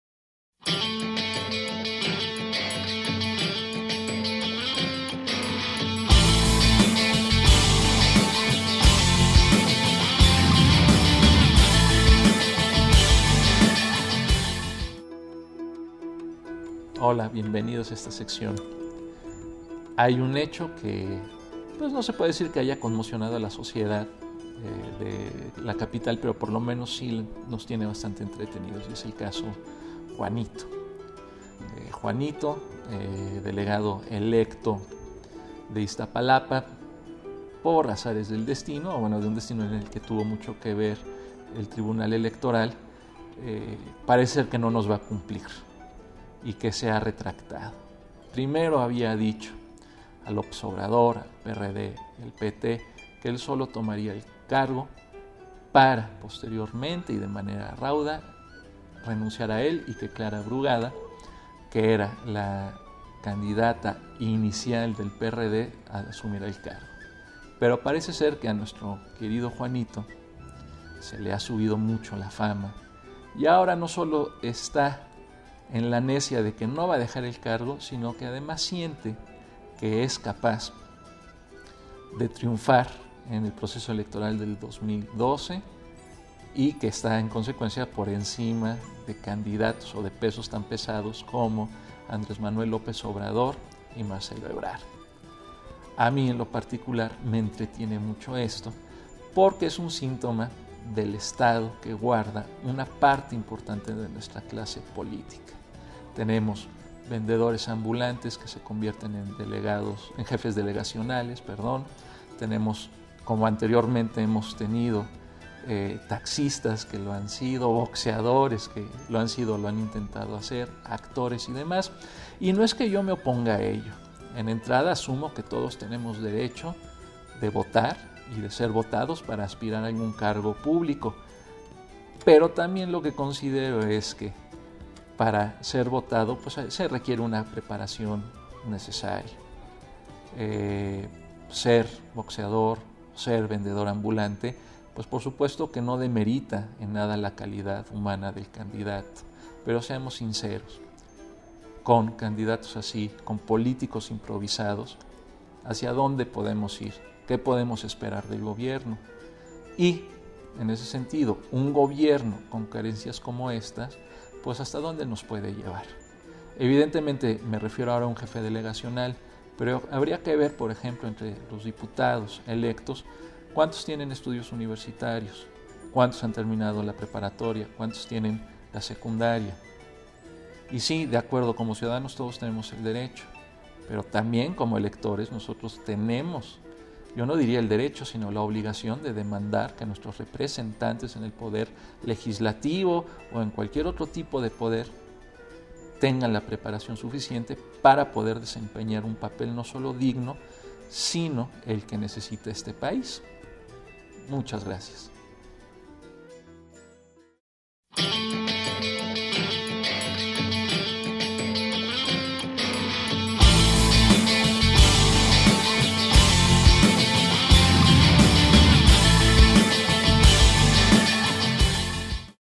Ahora cualquiera puede ser electo
El caso "Juanito" pone de manifiesto la pobreza política en la que se encuentra sometida una nación en donde, aparentemente, cualquiera puede tener un cargo de elección popular sin contar con la preparación necesaria.